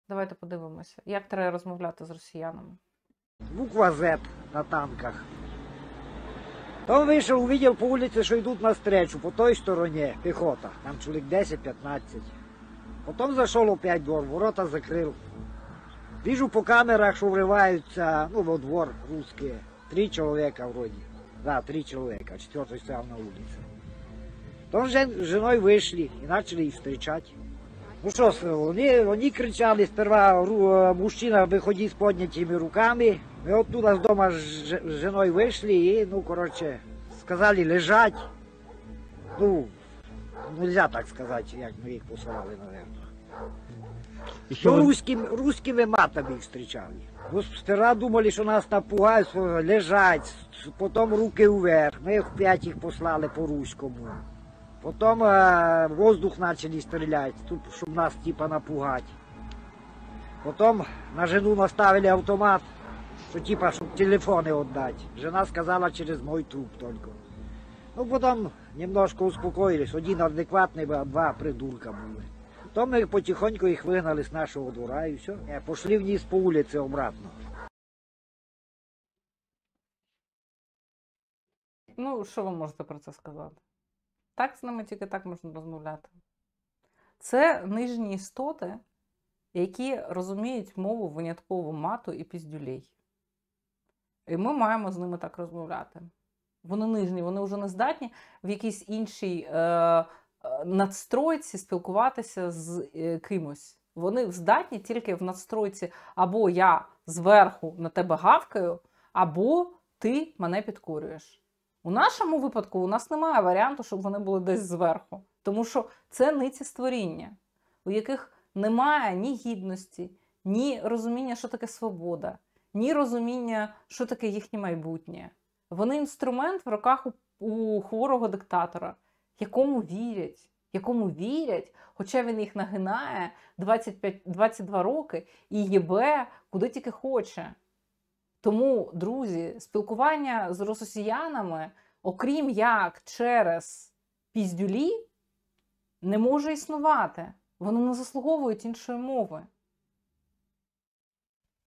Як спілкуватися із росіянами?🤔
Вознесенськ, Миколаївської обл. ✊🏻вкотре довів, що росіяни розуміють лише мову **люлєй.